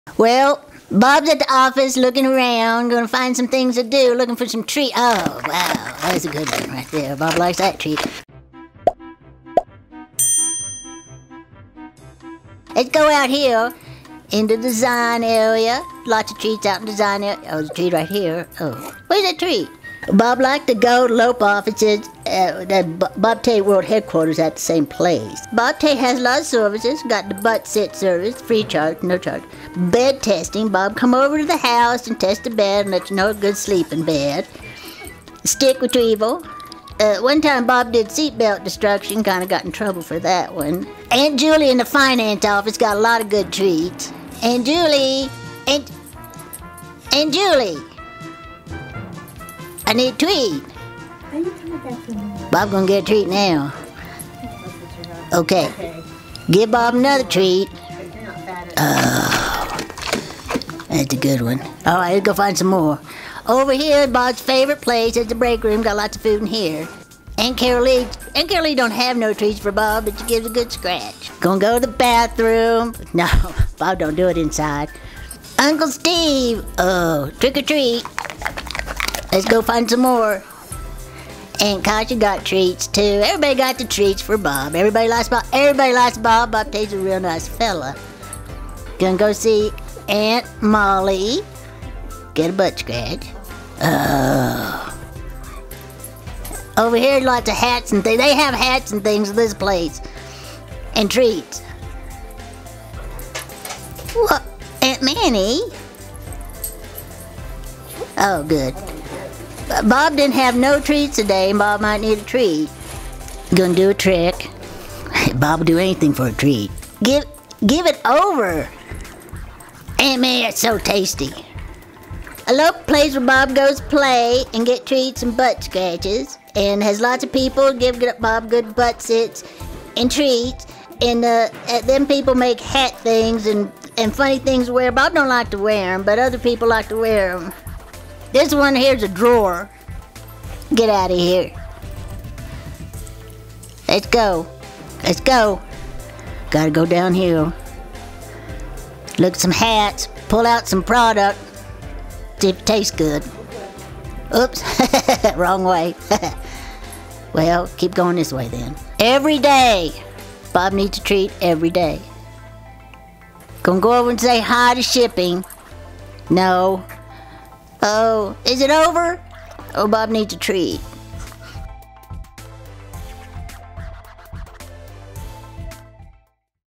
Bob Tate Tour
Bob Tate gives a tour of elope, inc. (and Bob Tate World Headquarters)